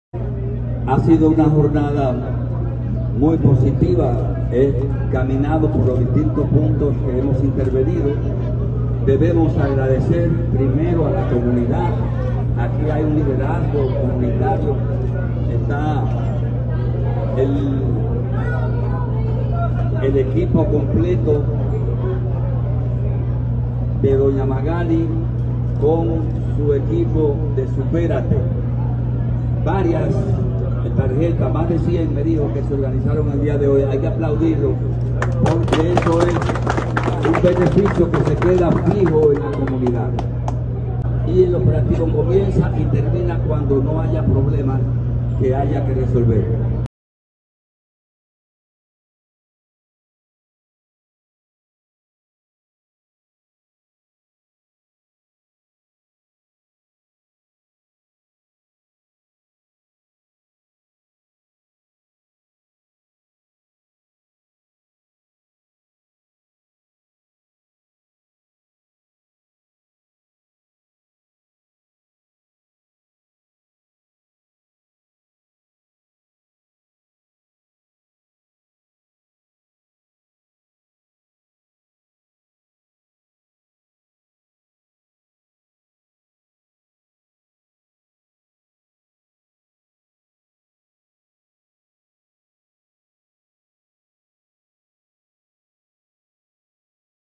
Alcalde Manuel Jiménez lleva El Despacho Municipal al sector Presidente Caamaño
ATMÓSFERA TV, SANTO DOMINGO.- El alcalde de Santo Domingo Este, Manuel Jiménez, continuó este fin de semana con los operativos de "El Despacho Municipal", esta vez en el sector Presidente Caamaño (Los Tres Brazos), donde desarrolló un amplio operativo médico, limpieza de cañadas, bacheo de calles y acondicionamiento de áreas verdes y de recreación.

Las acciones incluyeron, además, la entrega de raciones alimenticias, medicamentos, tarjeta Supérate, y atenciones en salud en las áreas de psicología, pediatría, ginecología y medicina general, donde decenas de niños y adultos se dieron cita a la escuela Santo Tomás de Aquino, donde fueron atendidos.